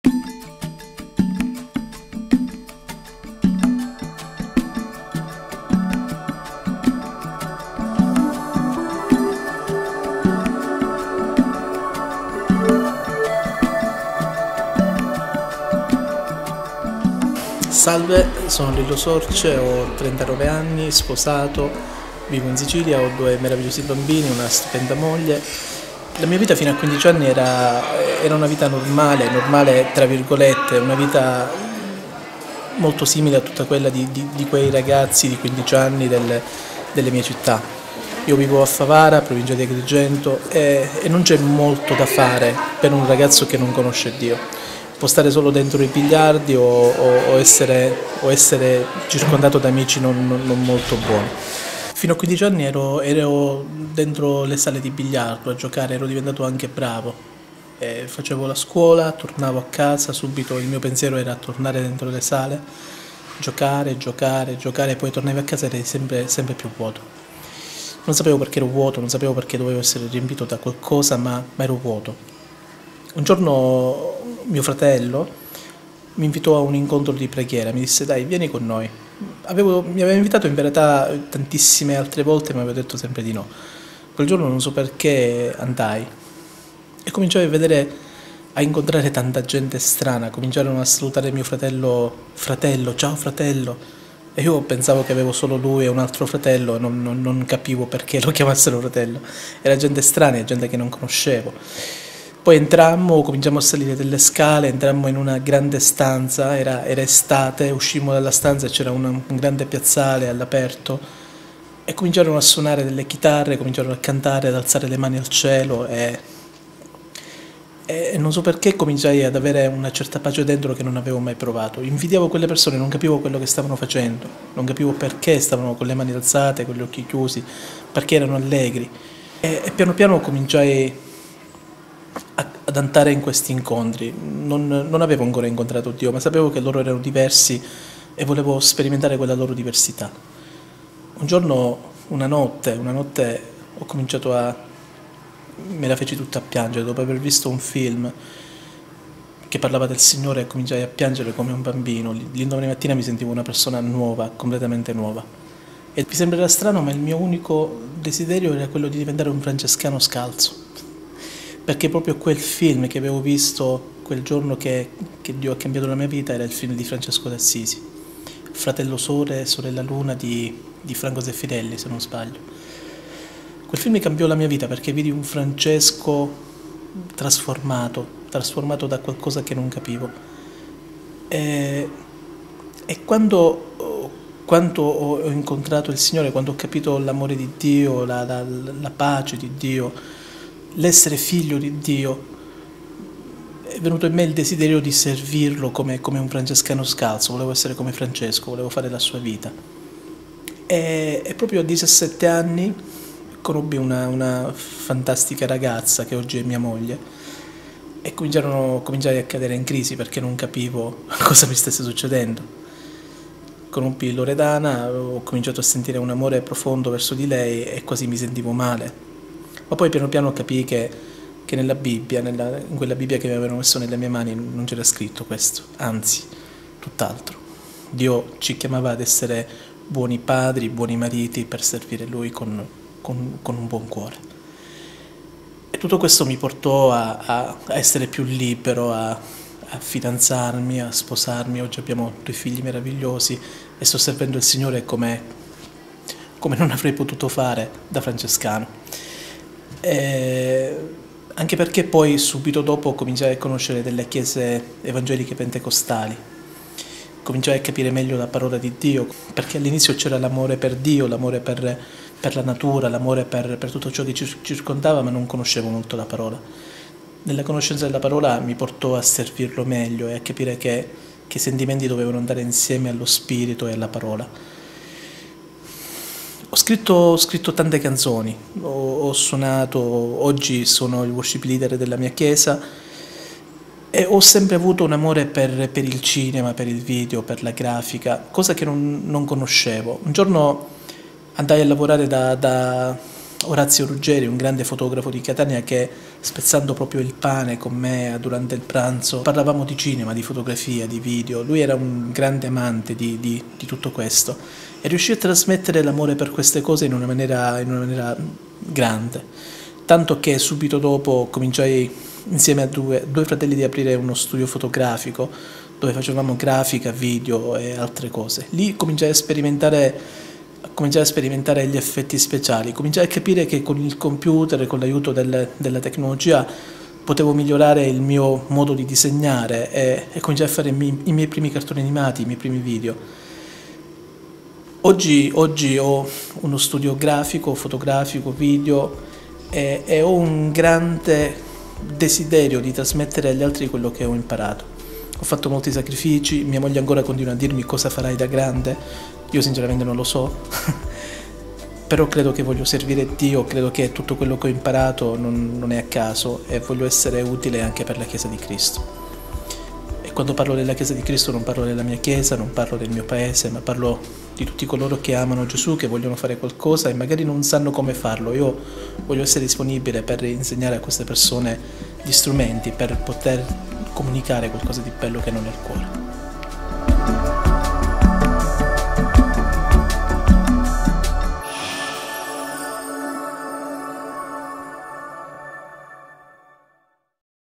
Volevo diventare un francescano scalzo...
Lillo Sorce, titolare dell'LSPictures, studio di produzione di effetti speciali per cinema e televisione, racconta la sua ammirazione per S. Francesco D'Assisi, il desiderio di seguirne le orme, l'incontro con Dio e lo stravolgimento della sua vita.